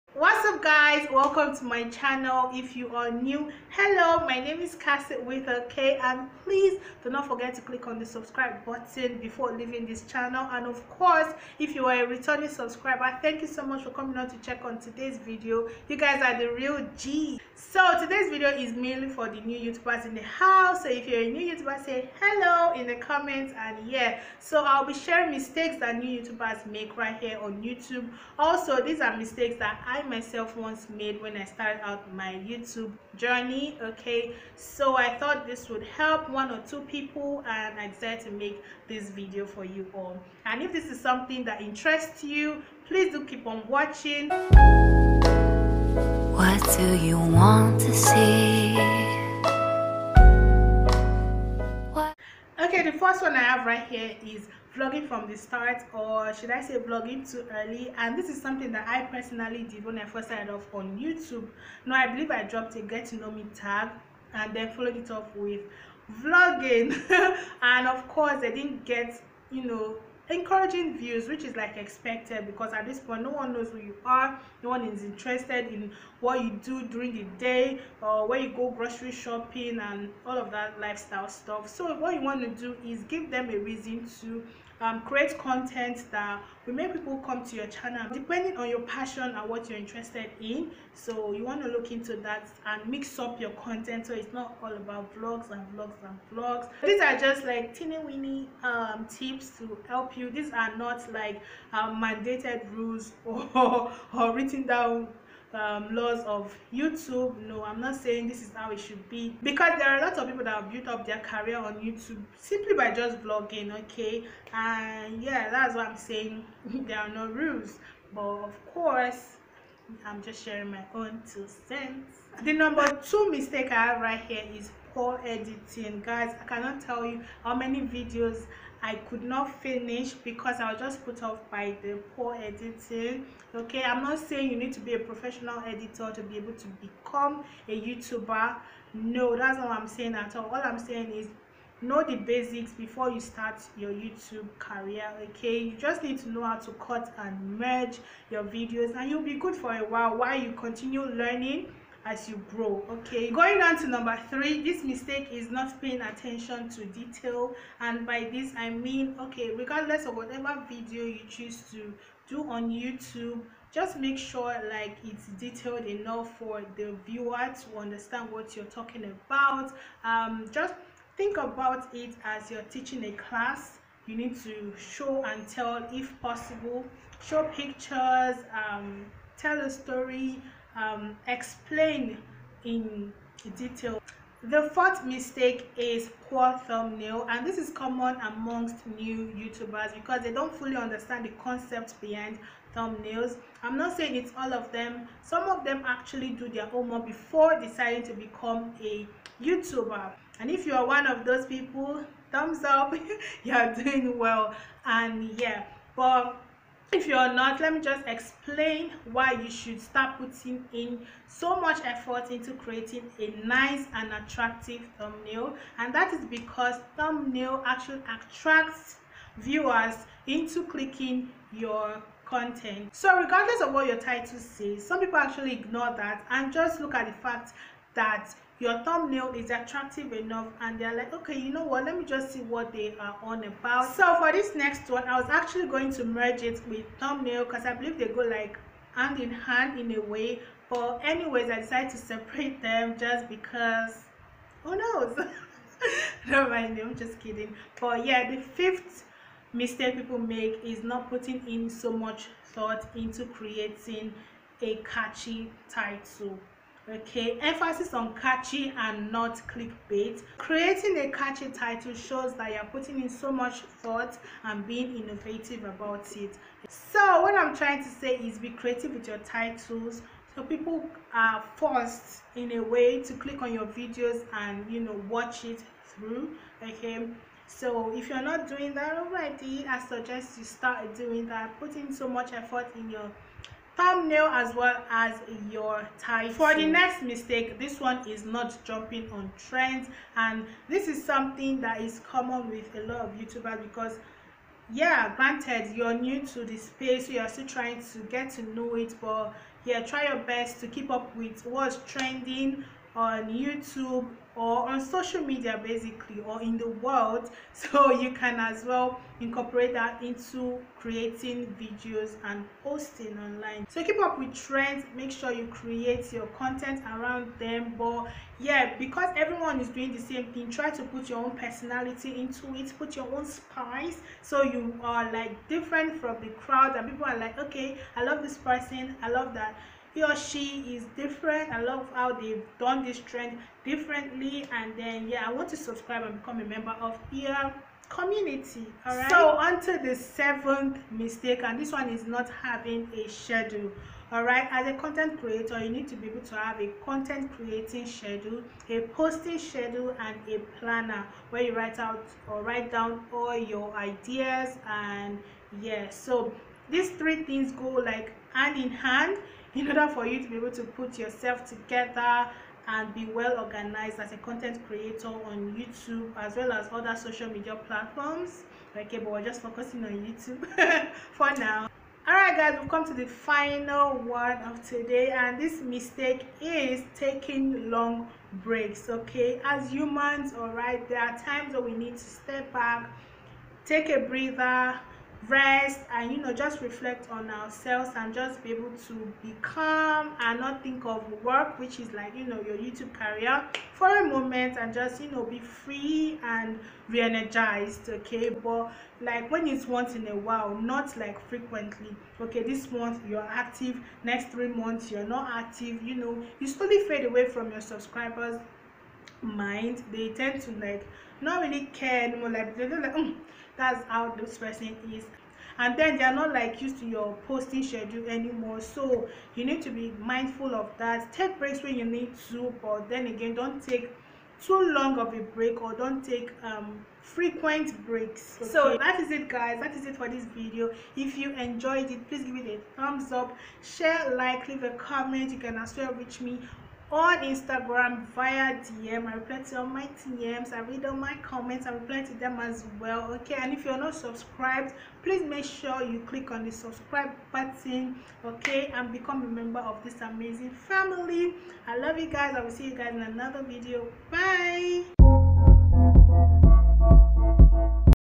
MISTAKES new YOUTUBERS make | Start your YouTube channel as a PRO
if you're new on YouTube and not sure of how to go about creating content then you definitely need to watch this video.